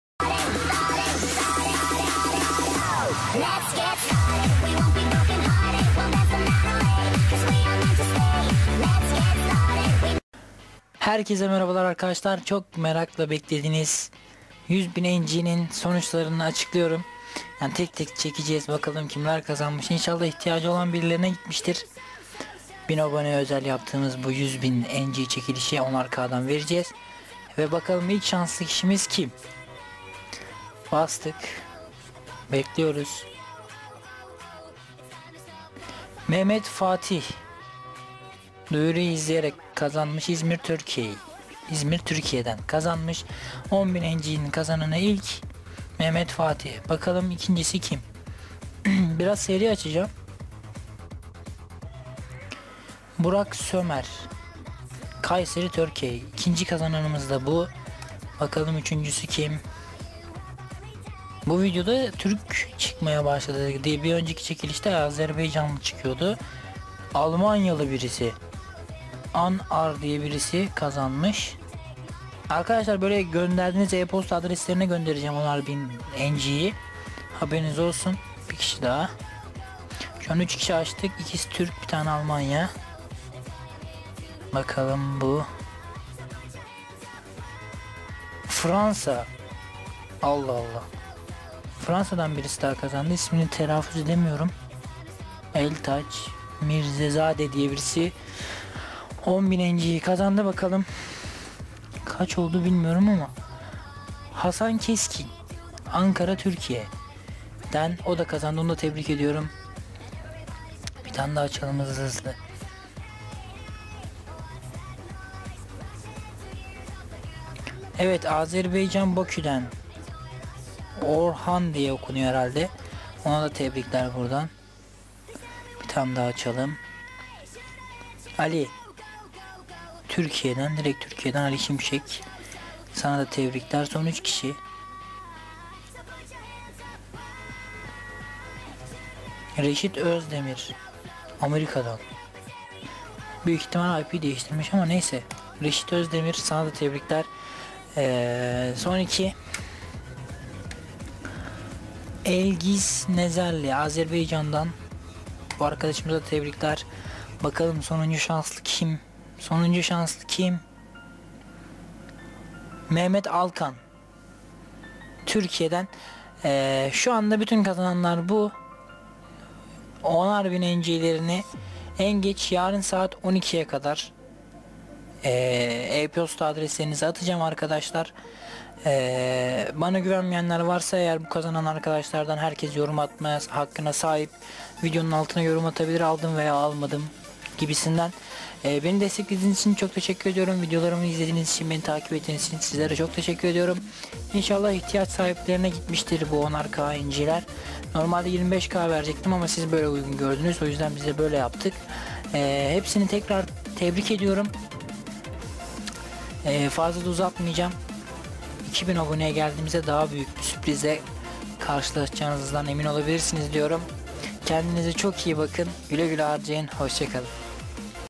100.000 NG KAZANANLARI^ TEBRİKLER#
1.000 Abone'ye özel yaptıgımız ng çekilişinin kazanan arkadaşlarını tebrik ediyorum. Güle Güle Harcayın.